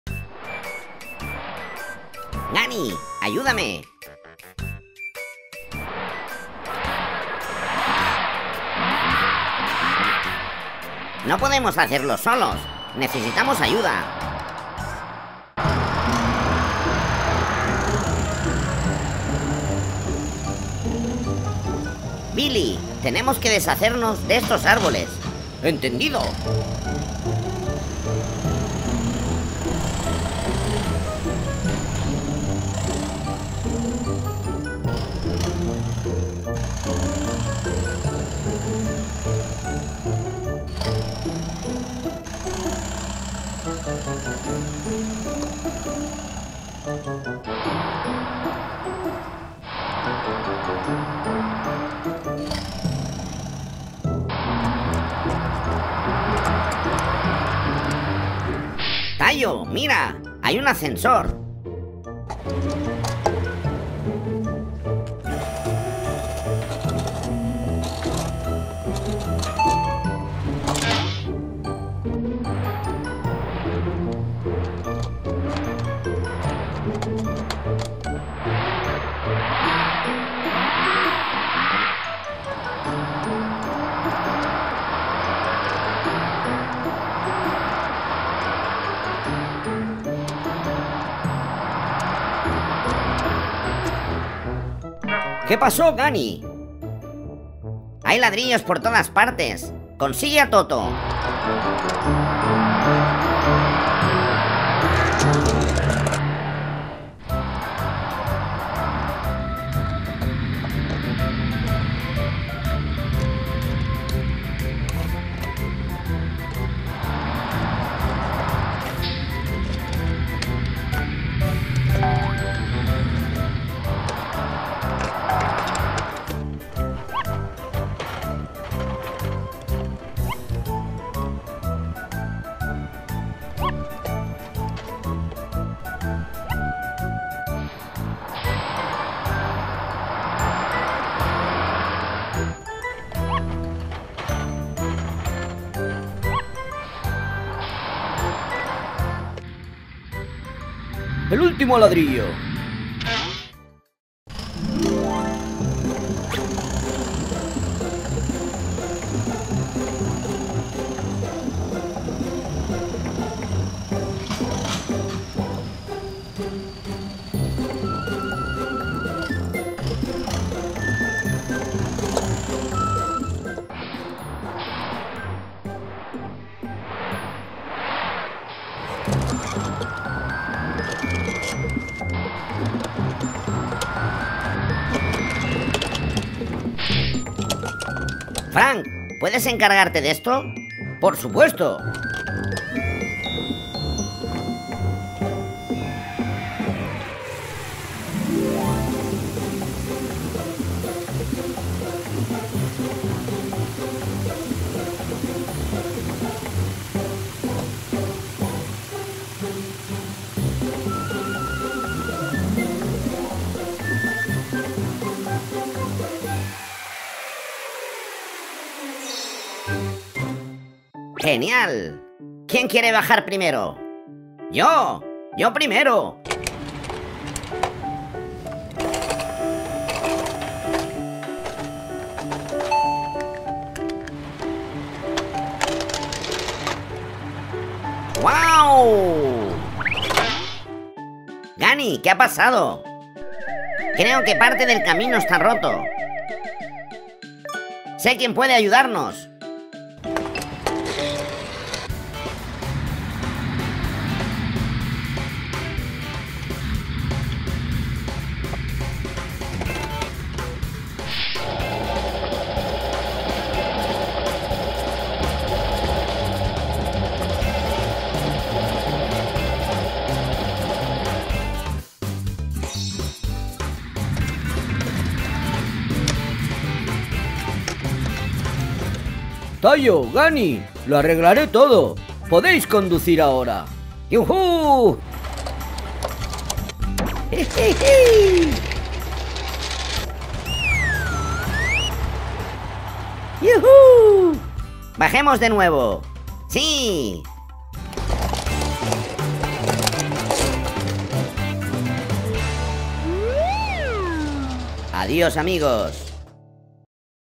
🚍 Tayo y Dani arreglan el aparcamiento. Aventuras de juguetes. Videos para niños.
Videos para niños de aventuras entretenidas. Los juguetes Tayo y Dani van al aparcamiento. Hay tantas cosas... restos de basura, troncos caídos y otras cosas que estorban, que los autobuses empiezan a limpiarlo. Necesitan la ayuda de su amiga, la pequeña excavadora. ¡Muy bien! Ahora van a subir a la última planta del aparcamiento en el ascensor. Veamos qué tal la bajada... Parece que hay un problema y hay que ponerle asfalto. ¡Nos ayudará una apisonadora!